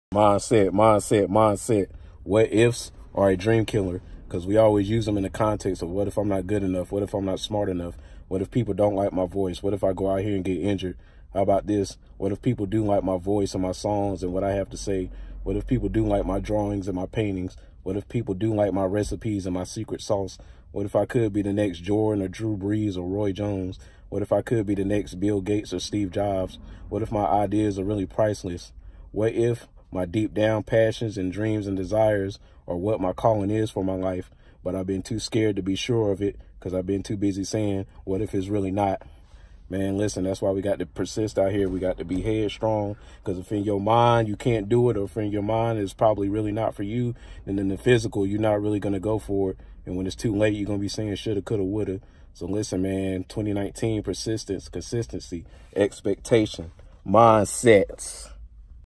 Mindset Segments: “What if’s”
“What if’s” can be a dream killer or they can be a dream builder, but they depends on your Mindset. Your mind is extremely powerful, spiritual even, and is a terrible thing to waste.